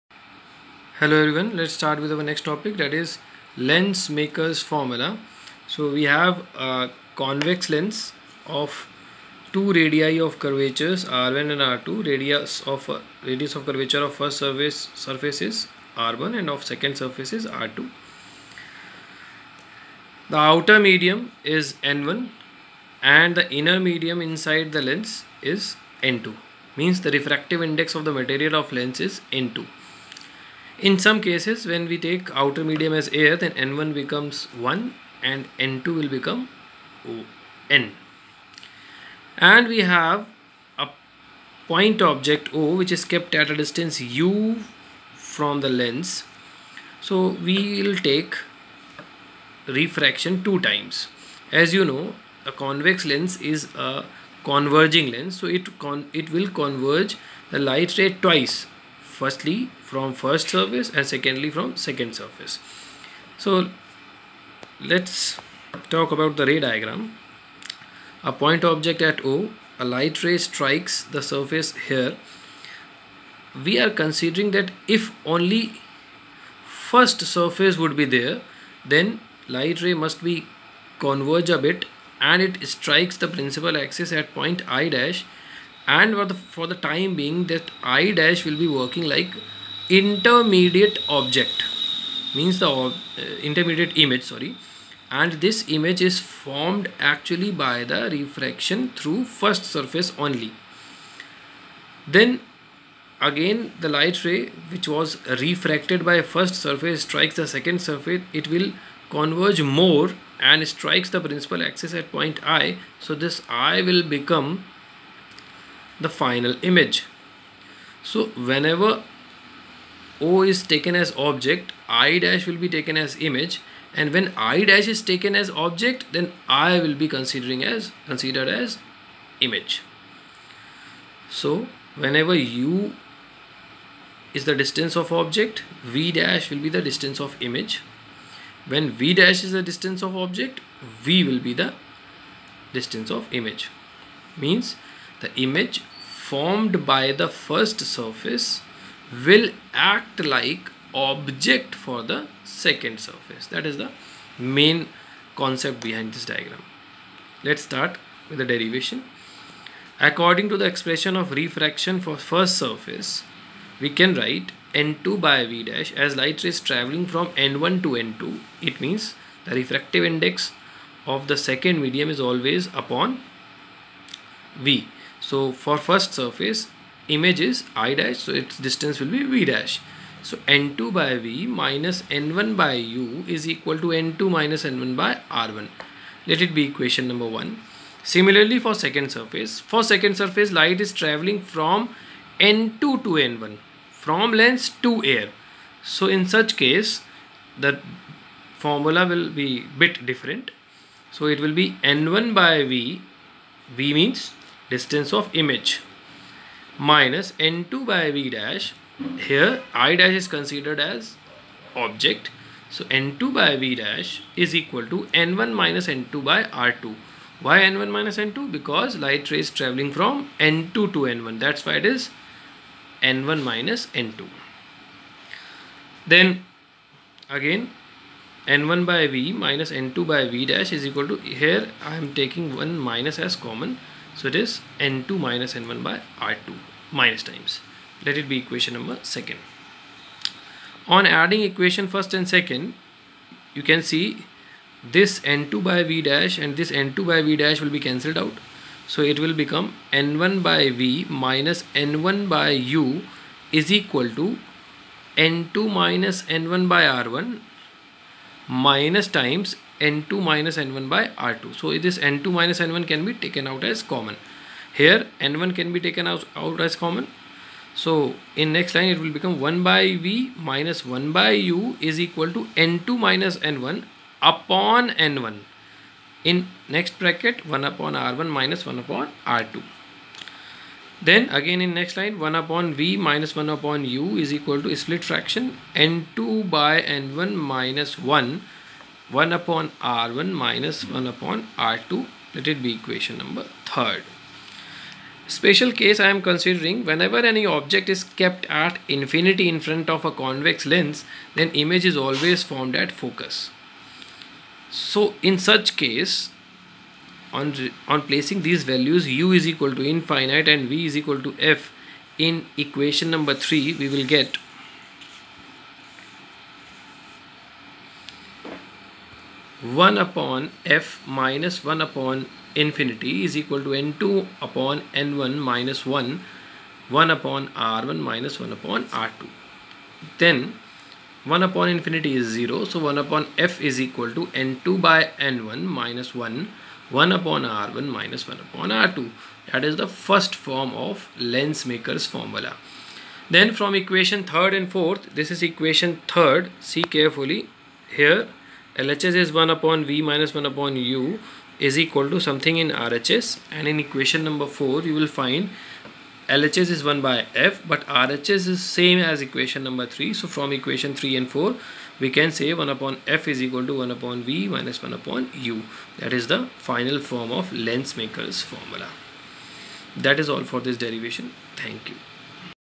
Lens Maker's Formula
Refraction through a Thin Convex Lens and derivation of Lens Maker's Formula